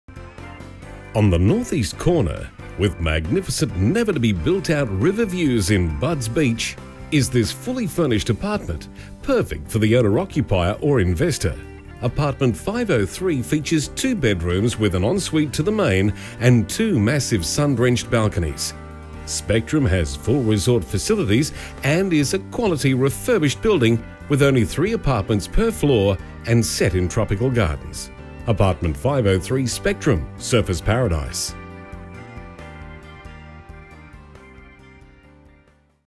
503 Spectrum 22 The Esplanade, Surfers Paradise QLD By Ross McVean
Property Video shoot of 503 Spectrum 22 The Esplanade, Surfers Paradise QLD by PlatinumHD for Ray White Surfers Paradise